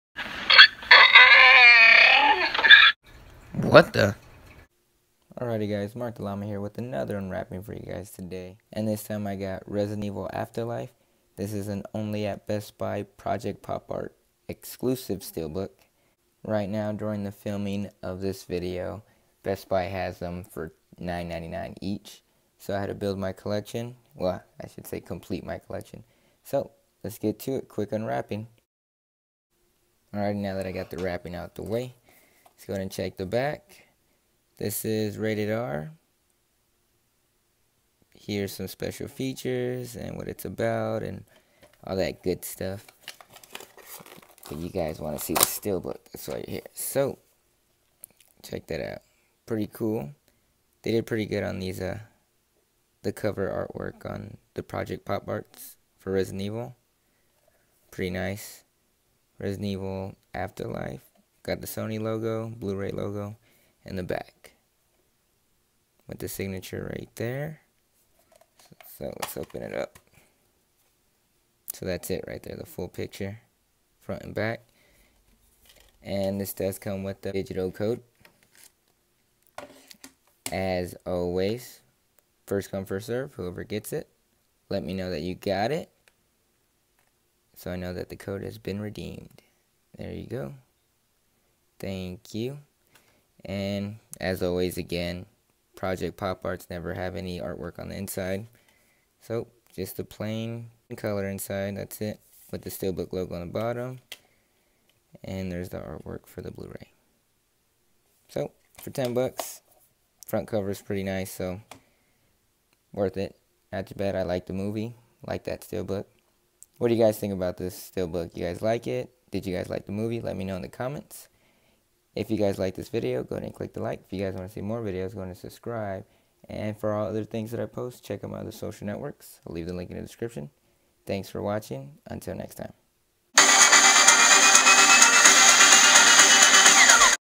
Resident evil Afterlife (Steelbook) project pop art UNWRAPPING/REVIEW! PLUS GIVEAWAY!
This is an only @ Bestbuy exclusive project pop art Steelbook. Comes with the Bluray, digital copy, Steelbook.